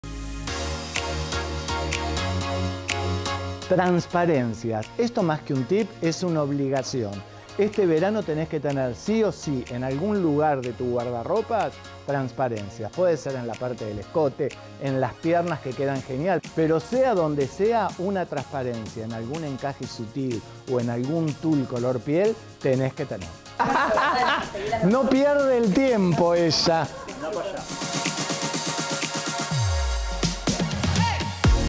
🌊TipsVerano: LAS TRANSPARENCIAS ¡Son casi una obligación!
¡Sí chicas! Claudio Cosano explica porqué más que un tip es una obligación.